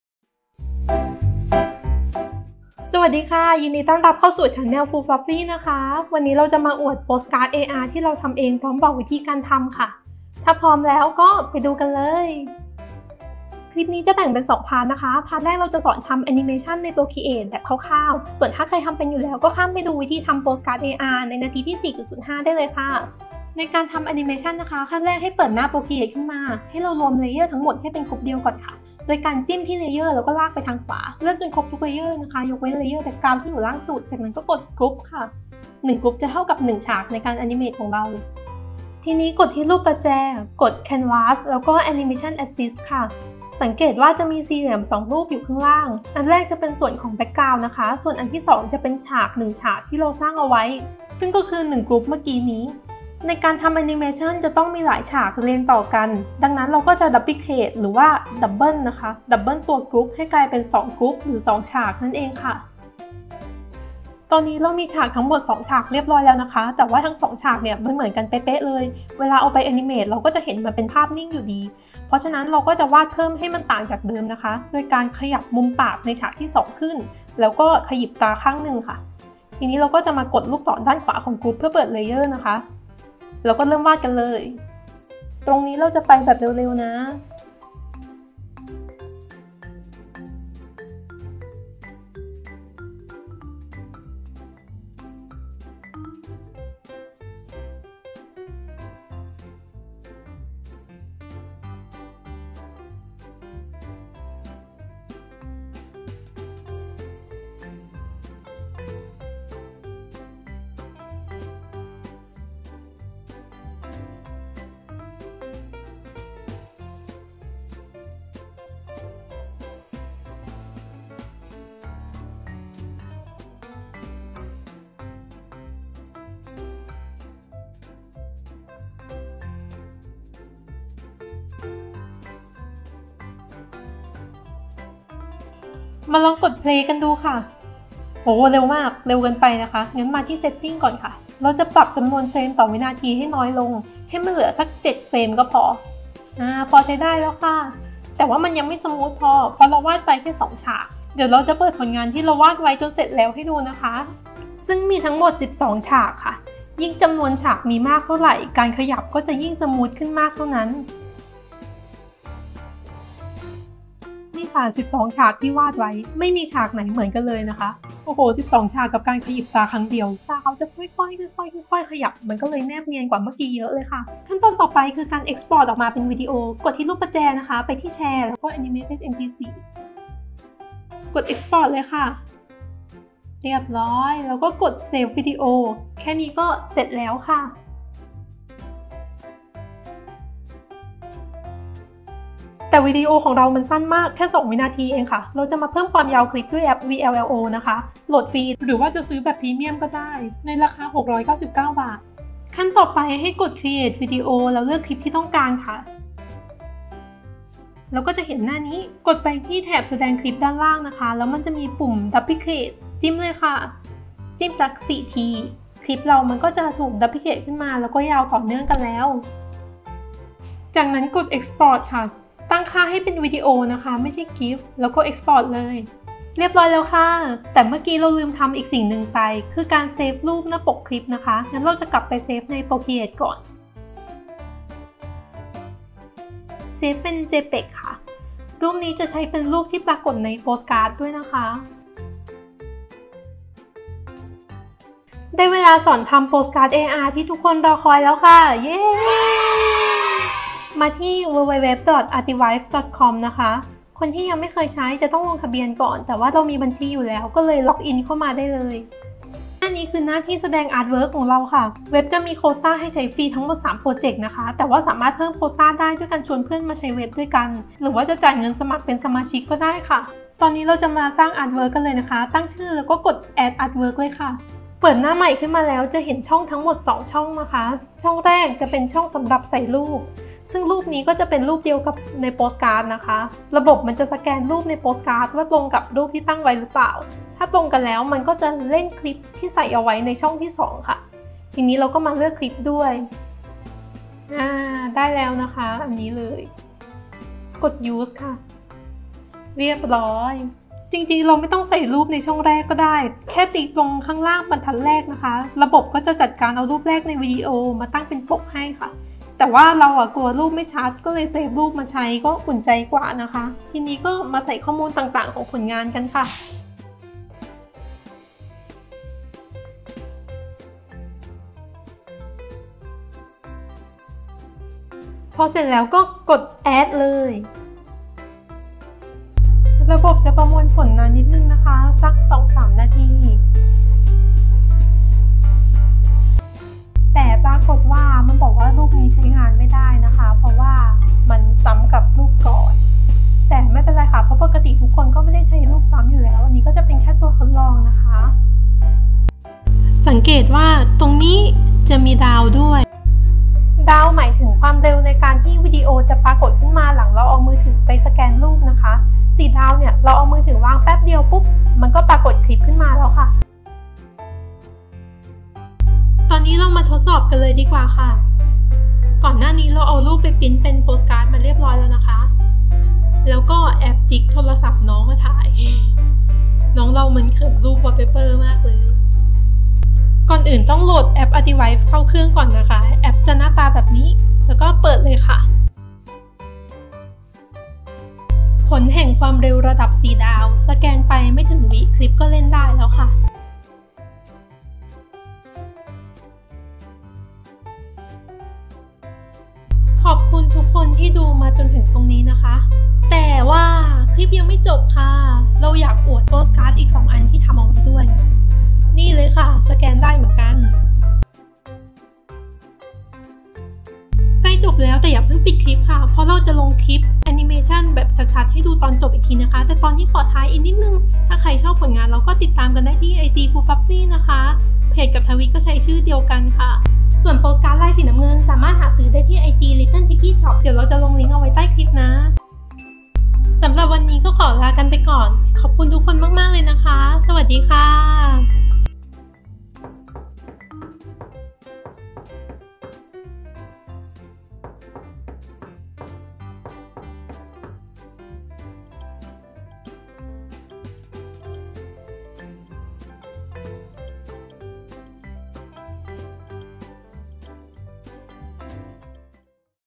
How to ทำโปสการ์ด AR ด้วย Artivive+Procreate l by Foofluffy
สวัสดีค่า เราจะมาสอนทำโปสการ์ด AR ฉบับรวบรัด
ชอบตรงไหนไม่ชอบตรงไหน คอมเมนต์มาบอกกันได้เลยน้าา